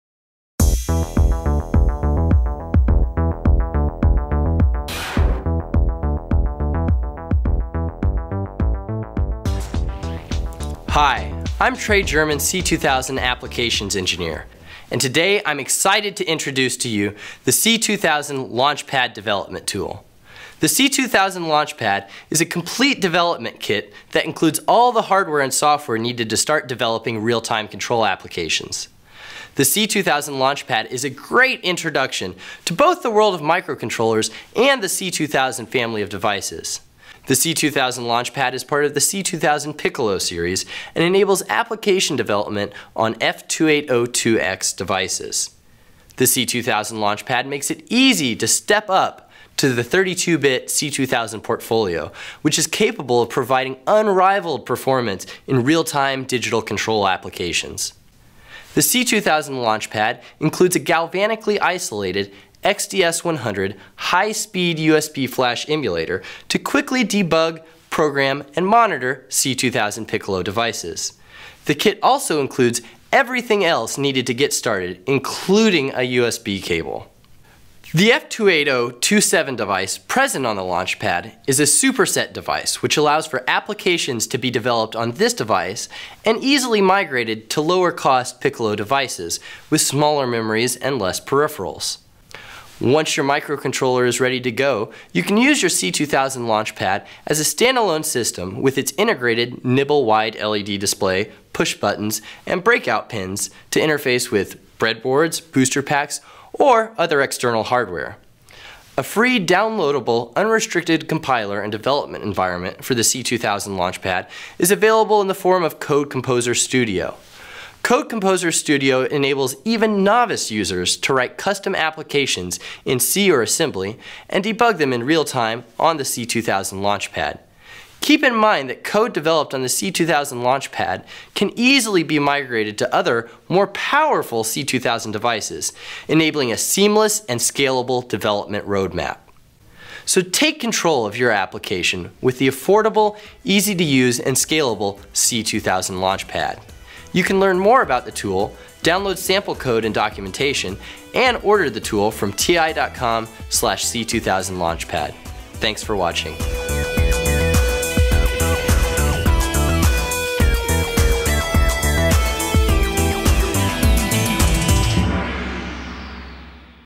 Discover the C2000™ LaunchPad™ Evaluation Kit
Based on the Piccolo™ 32-bit F2802x microcontroller, the C2000 LaunchPad enables developers, hobbyists and university students to explore the latest real-time control techniques for applications such as digital lighting and power conversion. This innovative, quick-launch evaluation kit contains everything needed -- device, emulation and software -- for development.  Developers can get started quickly with TI's free controlSUITE software and documentation.